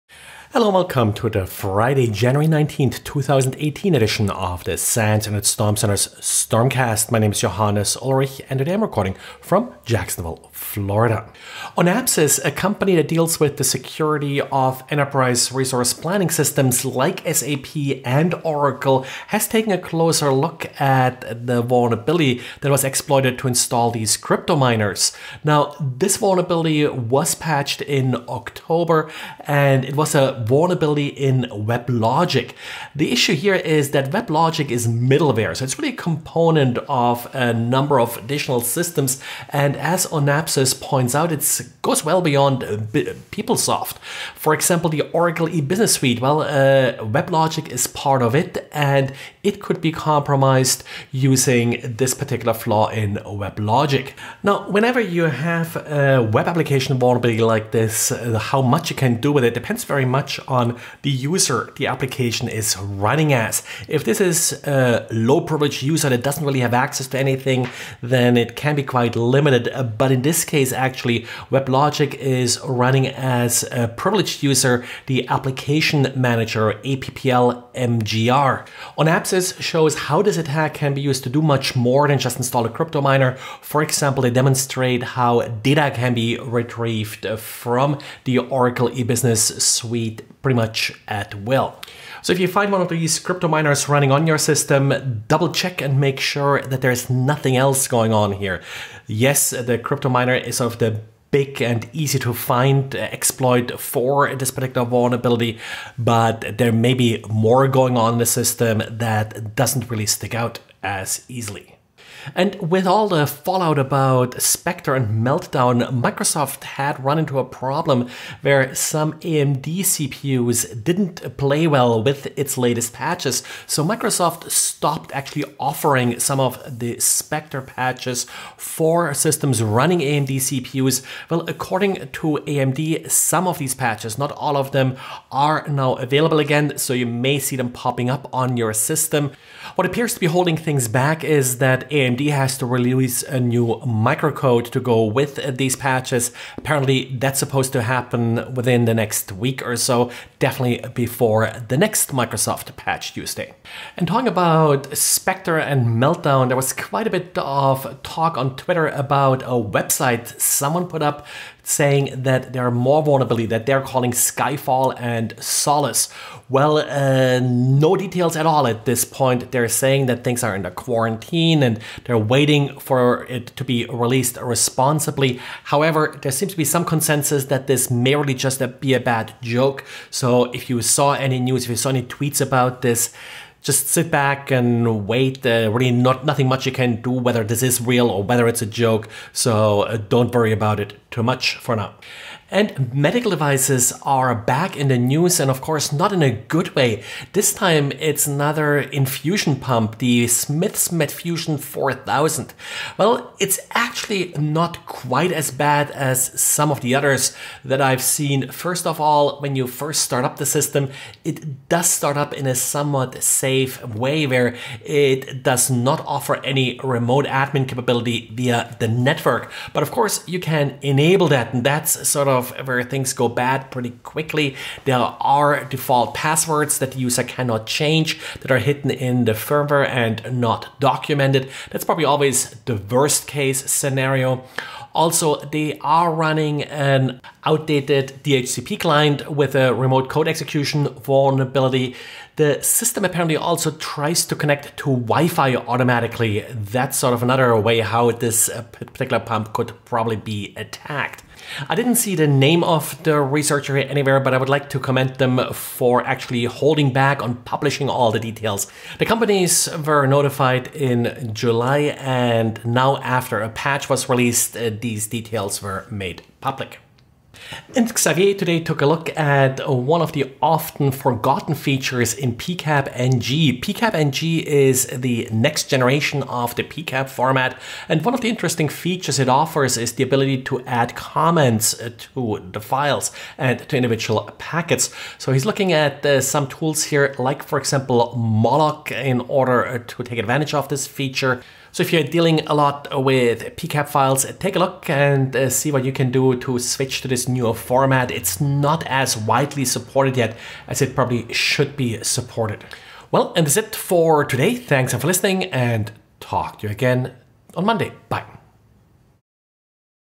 Network Security News Summary for Friday January 19 2018
Oracle EBS Vulnerable via WebLogic; MSFT Resumes AMD Patches; Infusion Pump Vulnerabilities

Oracle E-Business Suite Server Can Be Attackt via WebLogic